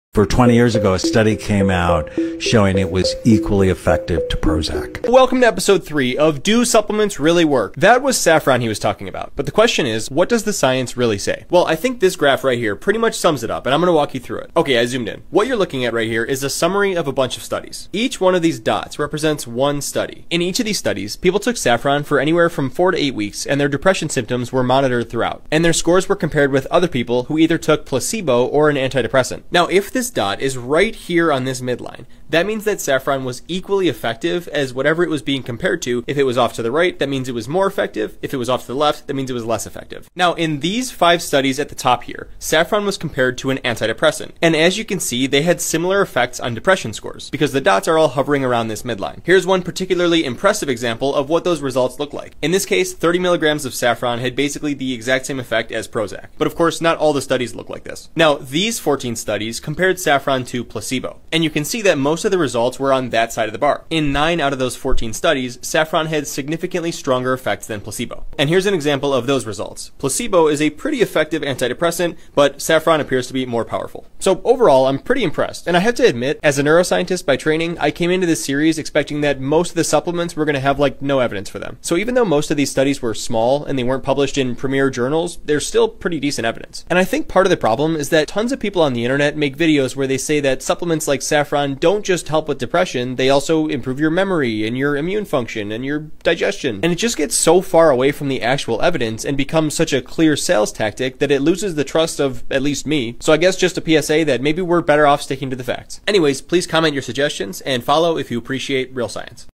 DO SUPPLEMENTS REALLY WORK? (Ep 3: Saffron) Today we’re investigating the question: does saffron
DO SUPPLEMENTS REALLY WORK? (Ep 3: Saffron)

Today we’re investigating the question: does saffron help with depression? Let’s turn to the scientific literature and find out what studies have shown.

Don’t forget to suggest supplements I should cover below. Thanks for your interest!